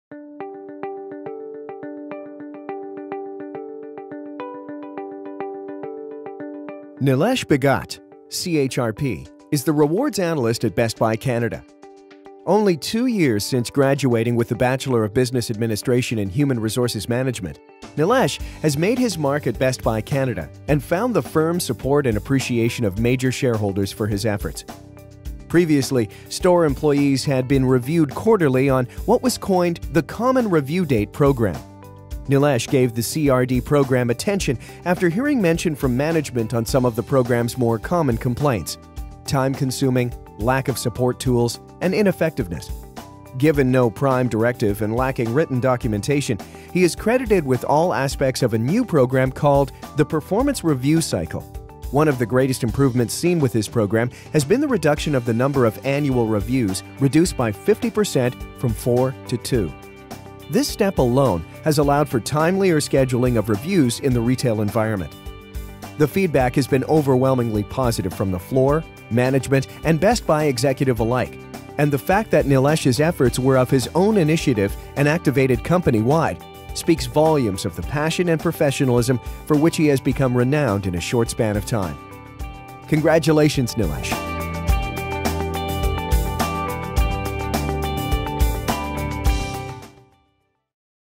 Nilesh Bhagat - Finalist for 2013 Rising Star Award
Nilesh Bhagat was a finalist for BC HRMA's 2013 Rising Star Award